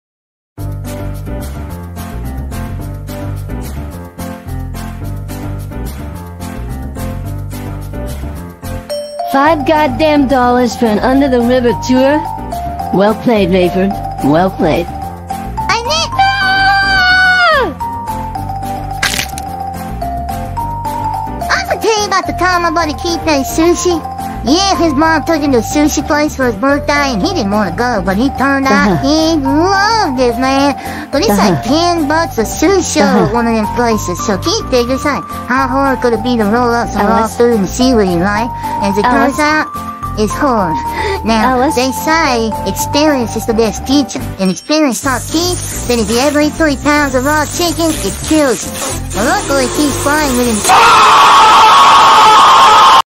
[MMDxHololivexL4D2] Miko's Stories (AI Voice)
Surely, Miko just want to storytelling about her adventures with Pekora, wouldn't you guys agree?

Animation made by me (took me 2 hours and half)
Rendering Video (10-20 minutes) and Editing (5-10 minutes)

RVC Models:
Hoshimachi Suisei (Shiro-Chan)
Sakura Miko (Aziib)

MMD Models:
Hoshimachi Suisei by TaraTarako
Newspaper by Feliks Laurinaitis